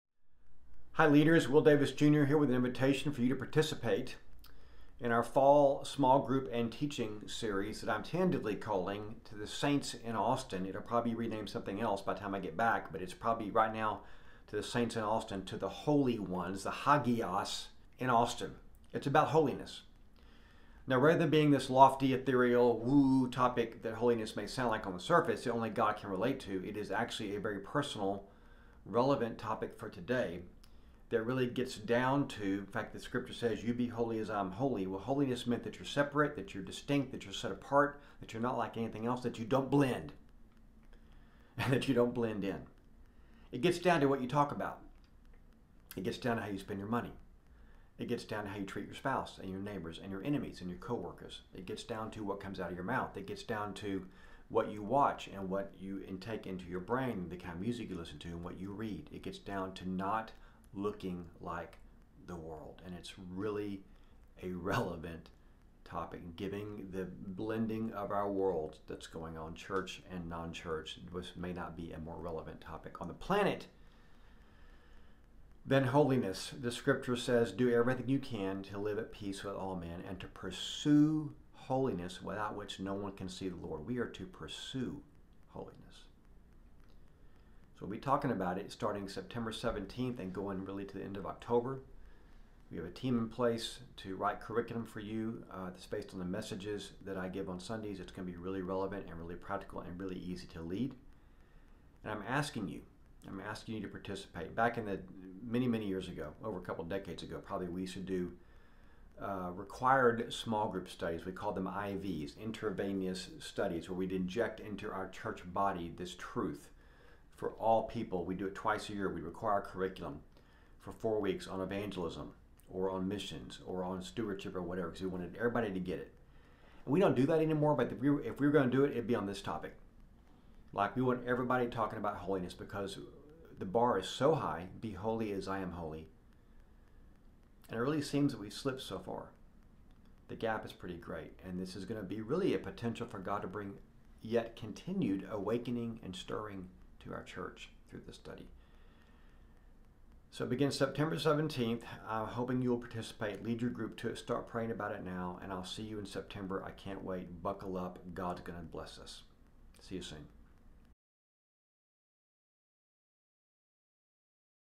Sacred Teaching & Small Group Series
Join ACFgroups for our fall study that will coincide with our Sunday morning teaching series called "Sacred." This series will focus on our holiness as Christ-followers and how we're called to be set apart. Join a group and access the study's resources and questions each week